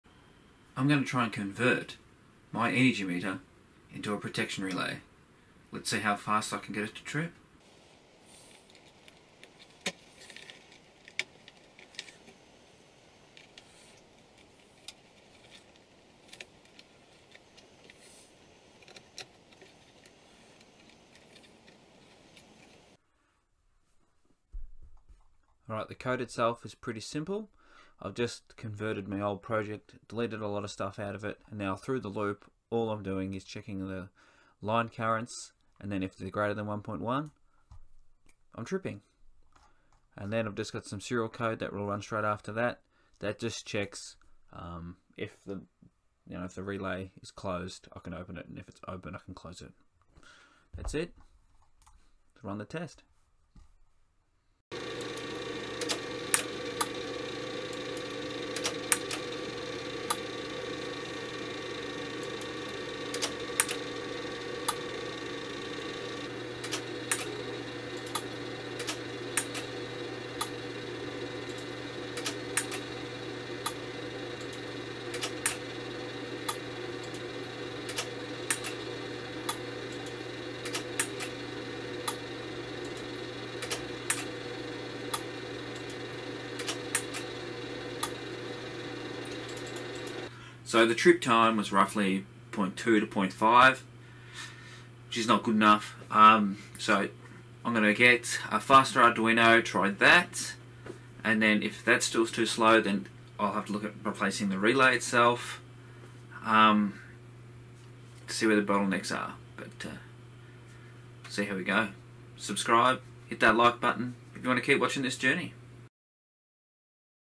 Arduino Power Protection Relay
Arduino mega using a Energy metering IC (ATM90E36) which I got from
tindie
Whatnick INC

Interested in more detail check out some of my other videos.